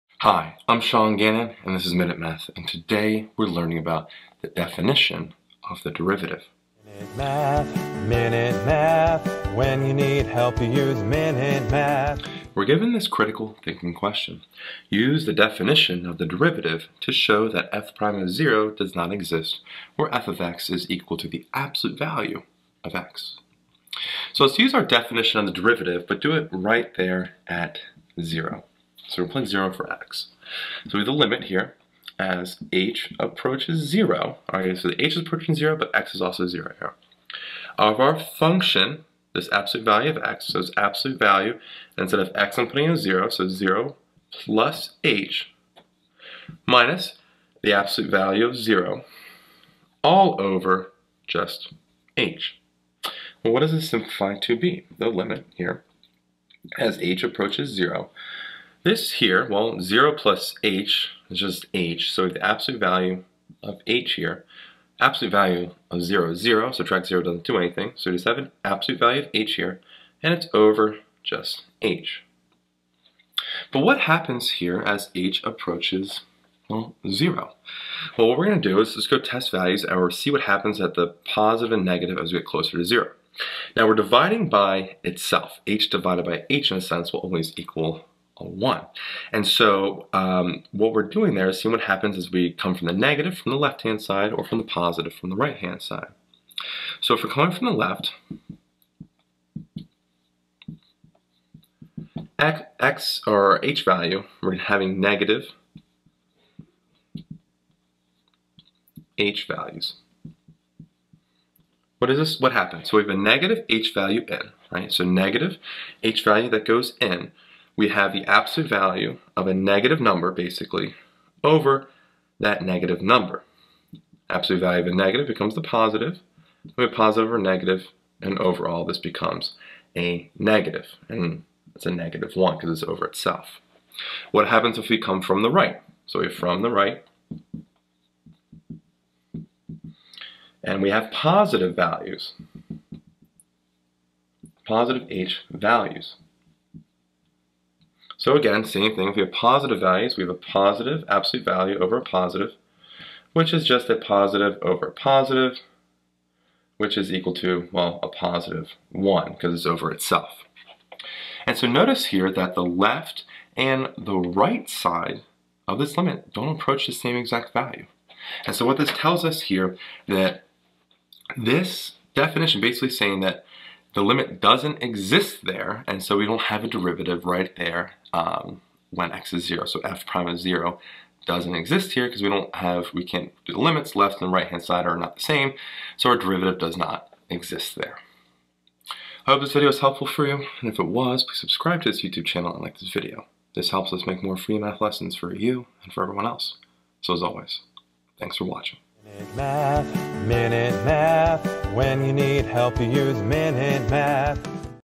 Use the definition of the derivative to show that f'(0) does not exist where f(x)=
How do you use the definition of the derivative? Use the definition of the derivative to show that f'(0) does not exist where f(x)=|x|. We learn how to use the definition of the derivative to show that f'(0) does not exist where f(x)=|x|. We will use limits to find the derivative of a function with respect to x with the definition of the derivative. This is a great introduction into derivatives. This is under the topic of Differentiation with this free online math video lesson.